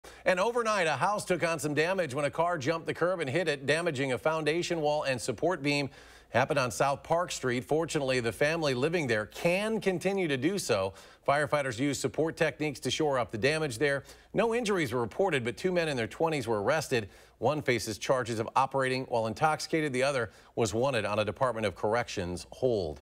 Two arrested after car crashes into Madison home
Madison police arrested two people early Friday after a car crashed into a house.